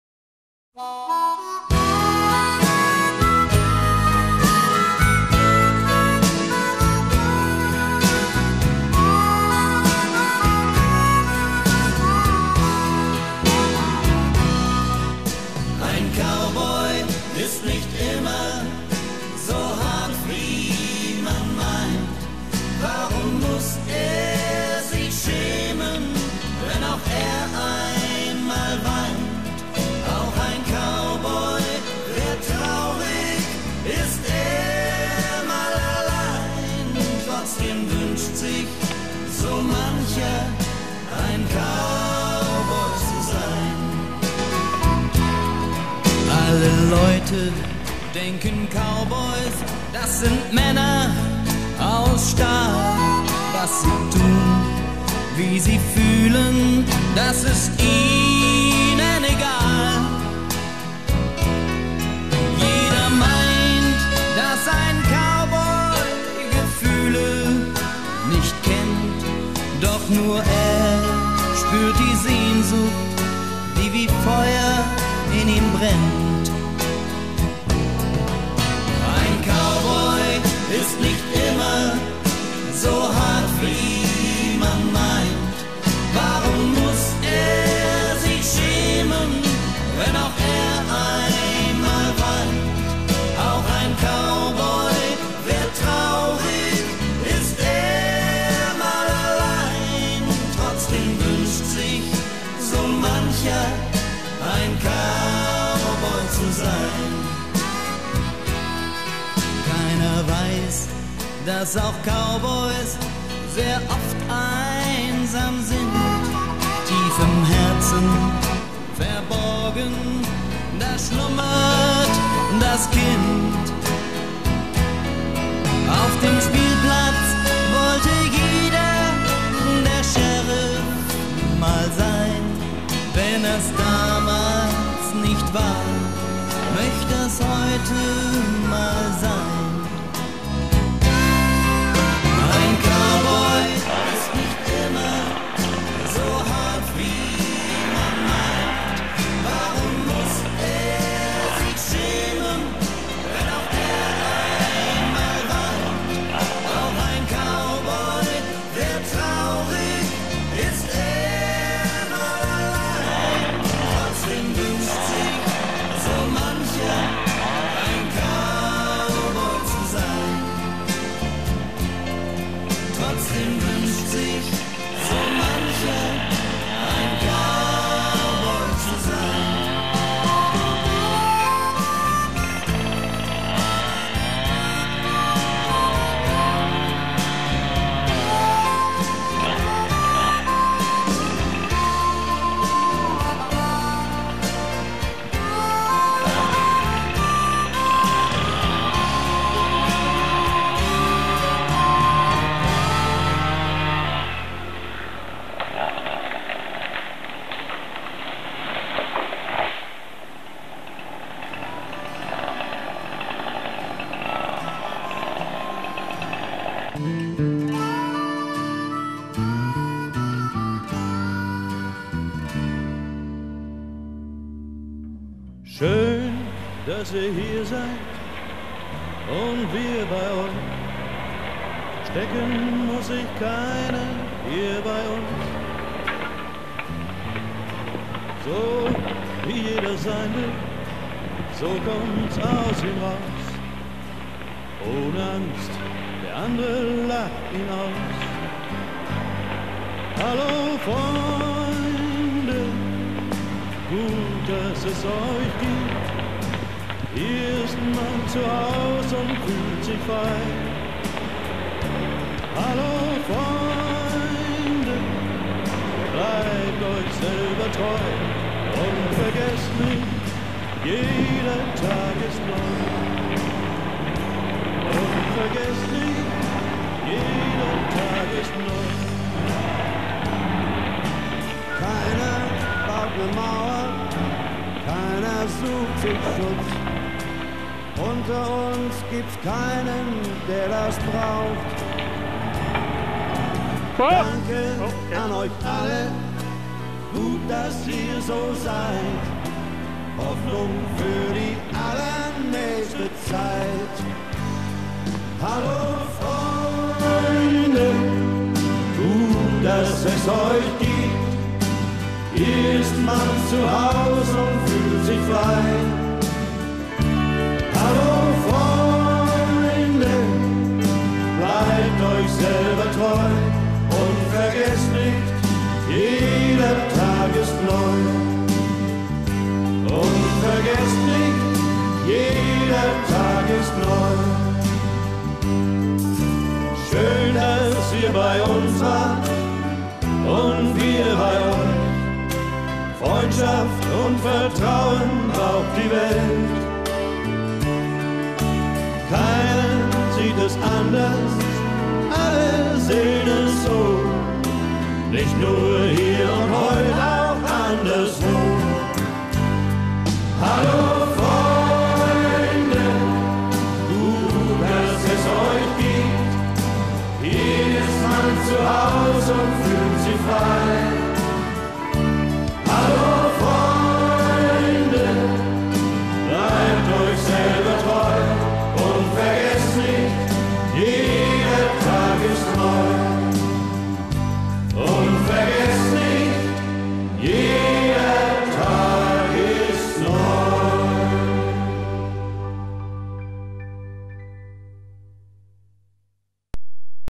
The Old Camper
The old camper was transported to his last resting place, hopefuly it will serve us as a hunting cabin for the years to come.
The songs are in German as this video was mainly produced for my german speaking friends and relatives, but do to demand the slide text was translated into english but I couldnt find any english songs to set the mood and have a meaningful text. Hope you still enjoy it.
Song one talks about Cowboys and that they are not always as tough as they seem and that they also cry once in a while when they are alone.
Song number two is about being thankful for friends and that where your friends are that is where you can  feel free and at home.